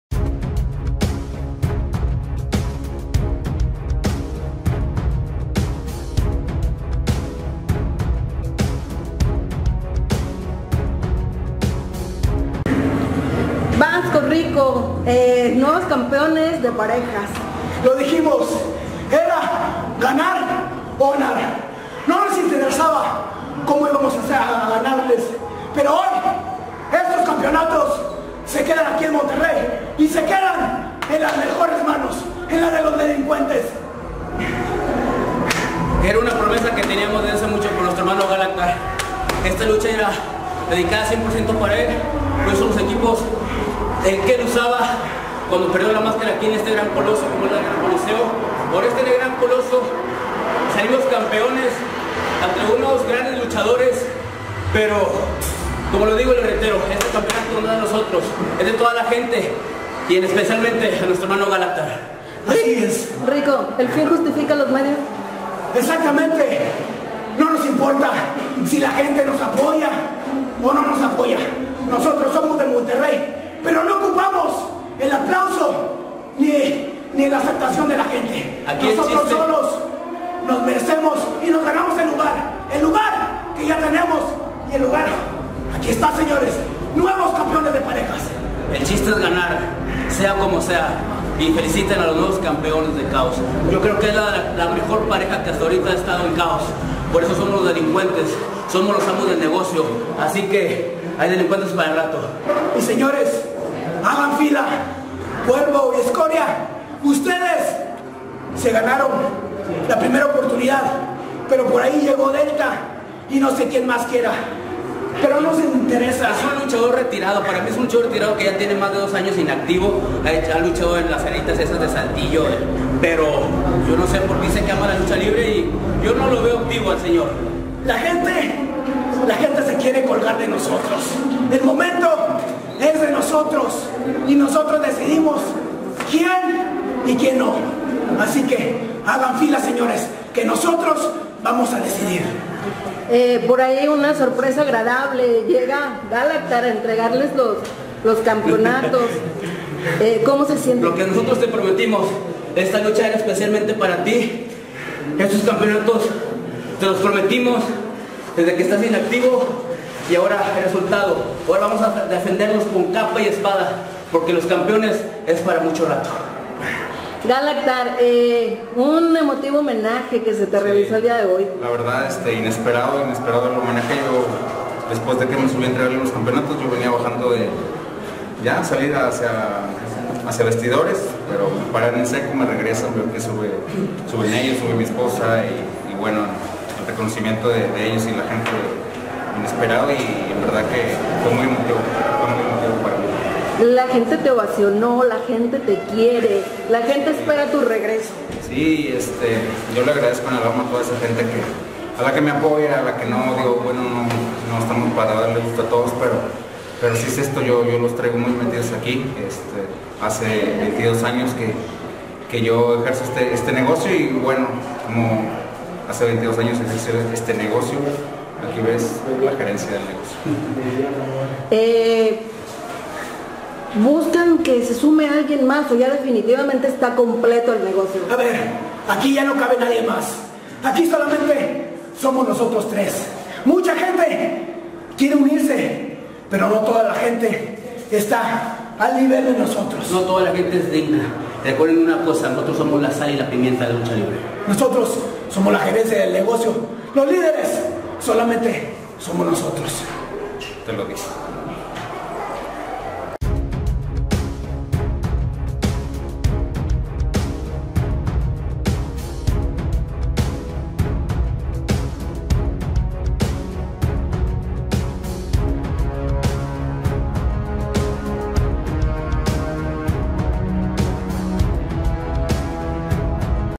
"Este Campeonato es de toda la gente y especialmente de Galactar" DELINCUENTES
El Vasco Jr y Rico Rdz son los nuevos monarcas de parejas de Kaoz Lucha Libre y los Delincuentes le dedican este Campeonato a Furor, estas las declaraciones de los Delincuentes.